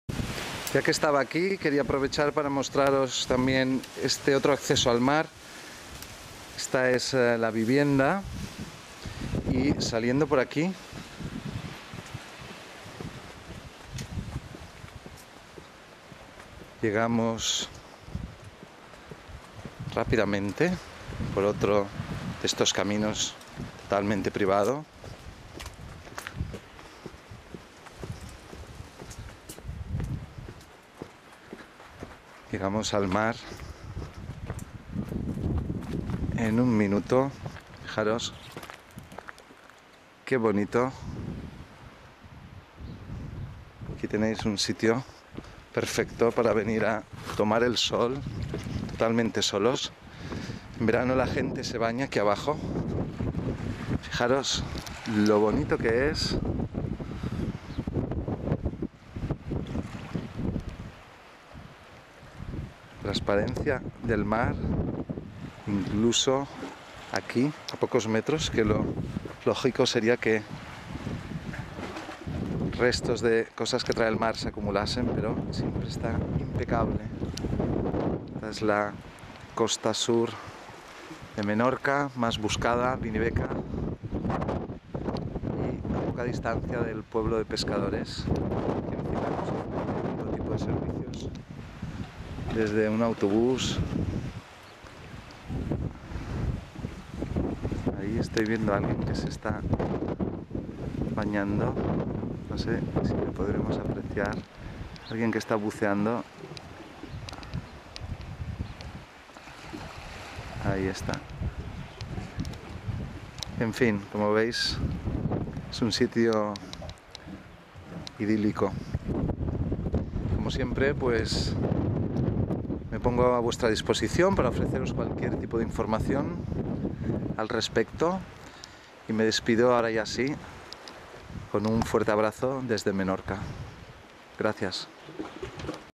Chalet en Binibeca Vell, primera línea (Parte II/II)
Segunda parte del vídeo de este fantástico chalet.
Alquila un casa como esta en menorca directamente del propietario

Second part of the video of this fantastic property.
Book a holiday home like this in Minorca directly from the owner on the link above